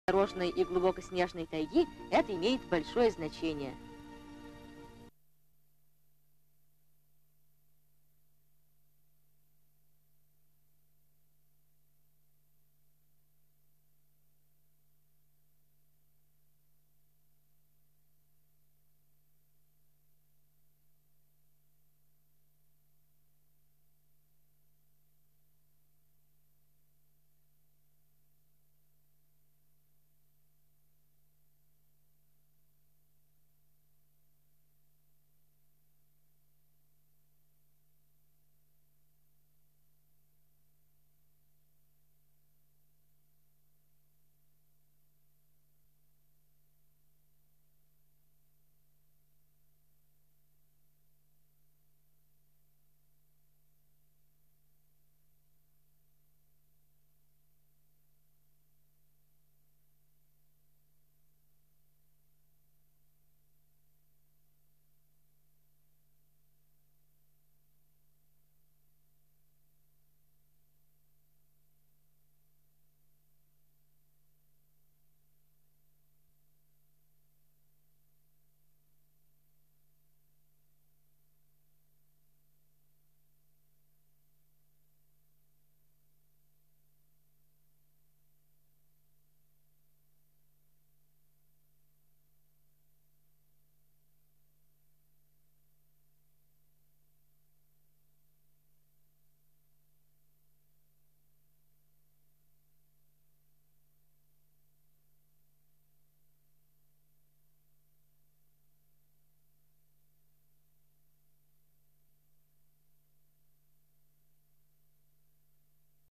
LONDON - B/W - NO SOUND
tilt angle gv of characteristic West End street of period, inc. Royal Institute Galleries on No 14 bus route to Putney - see next gv Leicester Square before it was pedestrianised - "Ben Hur" at the Empire, Monsigneur news theatre in bg, cars of contemporary models, some people looking at camera - finally, static shot of Dolcis shoe shop with "Sale" signs in windows, opposite Peter Robinson store - Oxford Street? Knightsbridge?